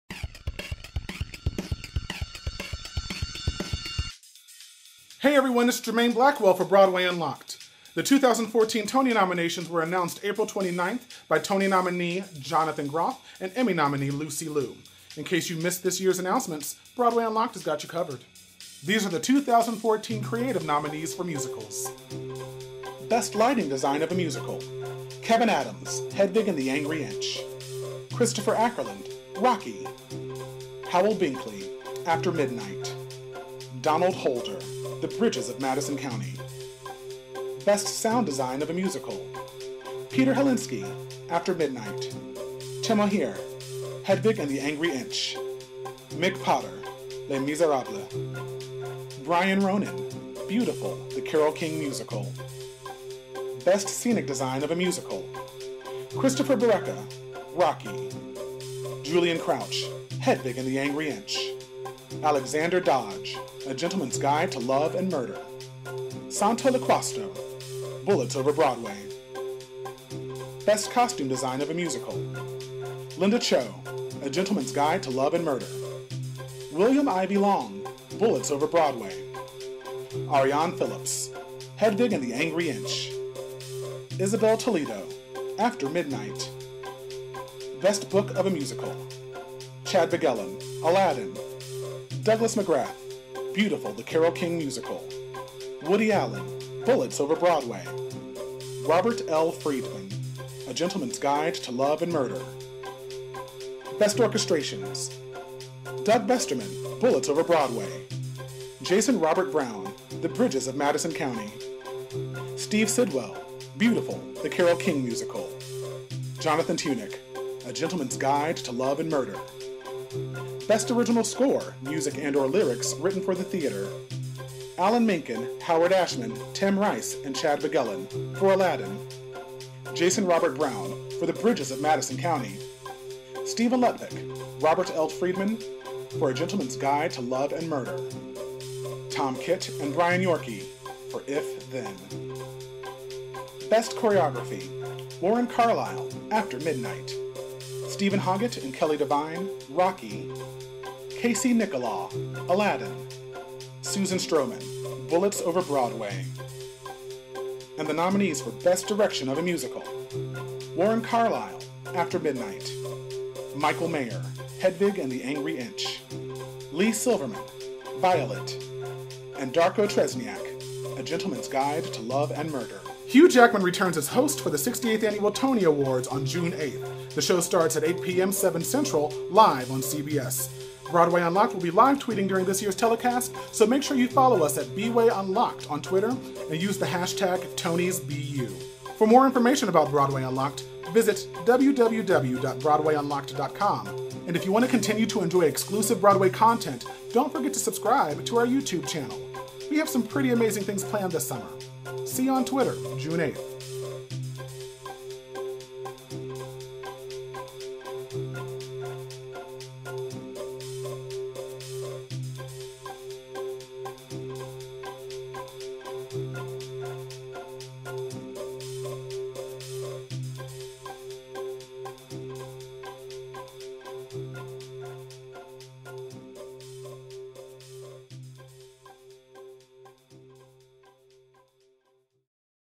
2014 TONY NOMINATIONS -Creative Nominees (MUSICALS) | broadwayUNLOCKED
2014 TONY NOMINATIONS -Creative Nominees (MUSICALS) | broadwayUNLOCKED

The 2014 Tony Nominations were announced April 29th by Tony Nominee Jonathan Groff and Emmy Nominee Lucy Liu.
Did your favorite performers get a Tony Nod? broadwayUNLOCKED has got you covered!

Hugh Jackman returns as host of the 68th Annual Tony Awards on June 8th. 8:00PM Eastern/ 7:00PM Central.
LIVE on CBS.

broadwayUnlocked will be Tweeting LIVE during this years Telecast.
broadwayUNLOCKED has some AMAZING plans for you this summer!